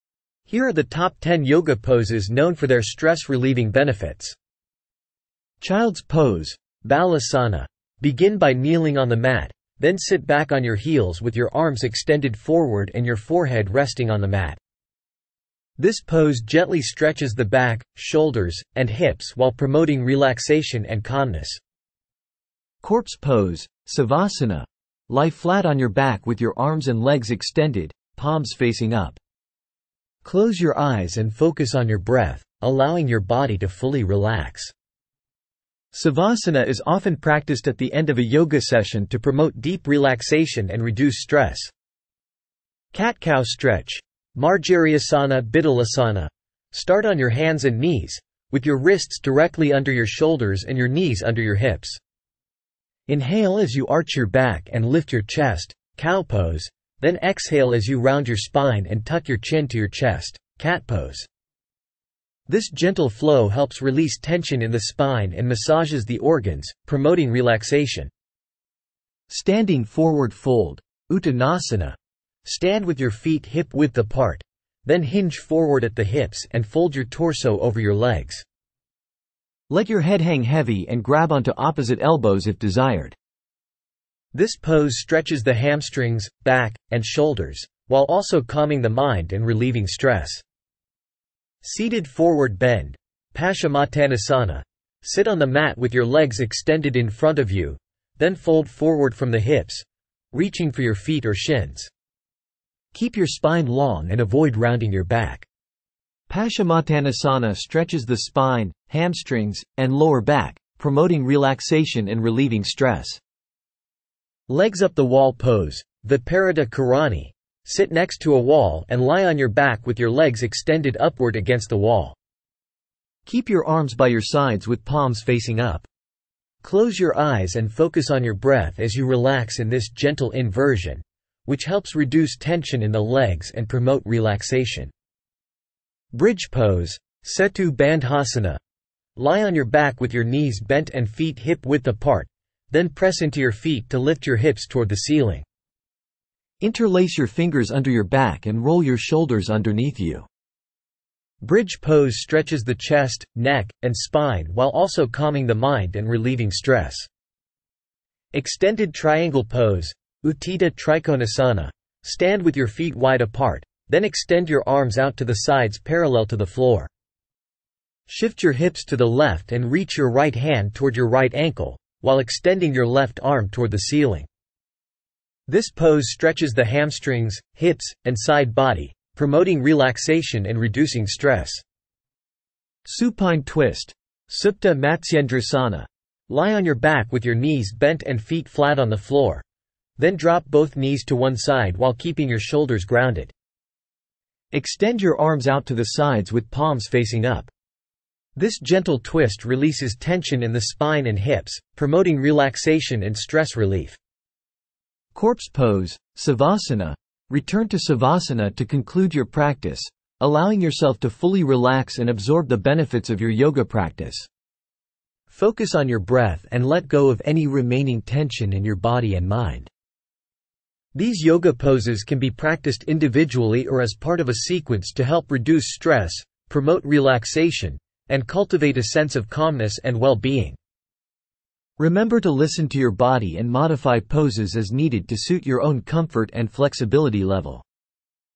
Top 10 Yoga Poses for Stress Relief
Find inner peace and relaxation with this selection of yoga poses designed to alleviate stress and tension. From gentle stretches to calming breathing exercises, discover the therapeutic benefits of yoga for promoting mental and physical well-being.